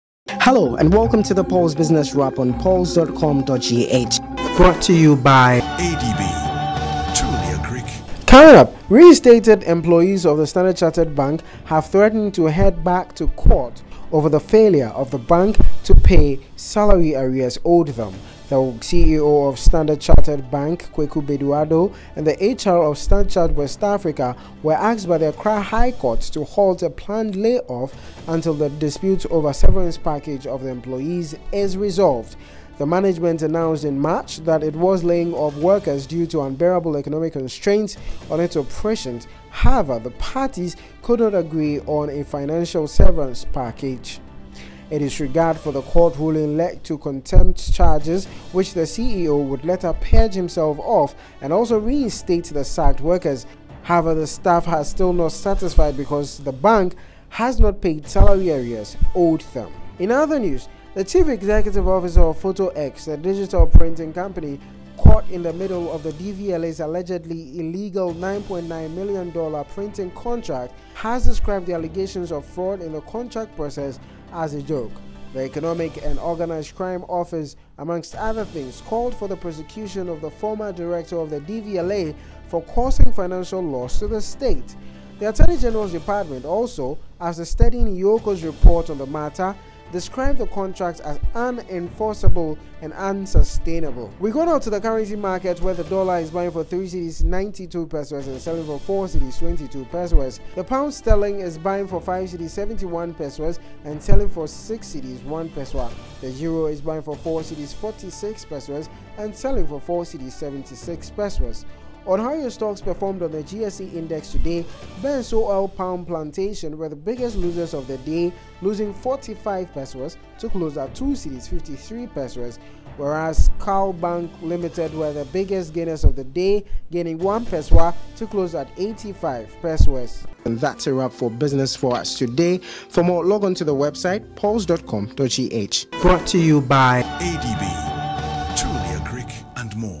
Pulse Business Wrap - 7th June, 2016
Foto X boss defends allegedly fraudulent DVLA contract

The Chief Executive Officer of Foto-X, the digital printing company caught in the middle of the DVLA’s allegedly illegal $9.9 million printing contract has described the allegations of fraud in the contract process as a joke.

The Economic and Organized Crime Office, amongst other things, called for the prosecution of the former director of the DVLA for causing financial loss to the state.

The Attorney General’s department, also, after studying EOCO’s report on the matter, described the contract as unenforceable and unsustainable.

In response to these allegations, however, CEO of Foto X Dr. Ian Kluvitse said the allegation by the AG’s Department “Is a big joke, and extremely laughable.”

Irate workers of StanChart to head back to court

A disregard for the court ruling led to contempt charges, which the CEO would later purge himself of and also reinstate the sacked employees.

However, the  staff are still not satisfied because government has not paid salary arrears owed them.

After the contempt hearing on Monday,  lawyer for the staff, Prince Nii Ashie-Neequaye gave management of Standard Chartered Bank the deadline of  June 20, 2016,  to pay back the arrears of his clients, or be hurled back to court.

We go to the currency market
Currency                      Buying                         Selling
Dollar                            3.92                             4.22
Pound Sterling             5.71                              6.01

On how your stocks performed on the GSE Index today
BOPP were the biggest losers of the day, losing 45p to close at 2.53…whereas Call Bank were the biggest gainers of the day, gaining 1p to close at 0.85p.